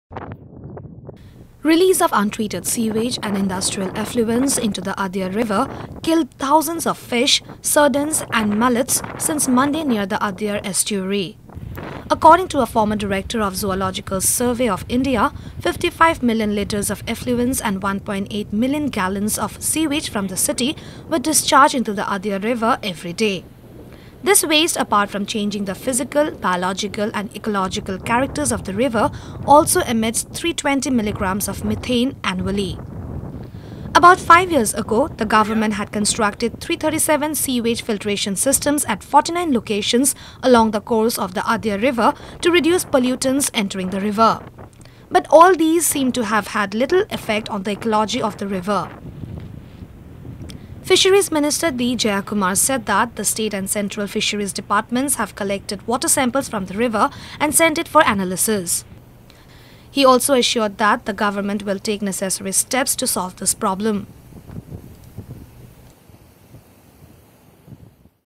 Chennai: Thousands of fish killed as sewage released into Adyar river
Thousands of fish were killed after untreated sewage and industrial effluents were released into the Adyar river in Tamil Nadu. According to a former director of Zoological Survey of India, 55 million litres of effluents and 1.8 million gallons of sewage from the city were discharged into the Adyar river every day. Meanwhile, fisheries minister D Jayakumar said that the state and central fisheries departments collected water samples from the river and sent it for analysis. He also assured that the government will take necessary steps to solve this problem.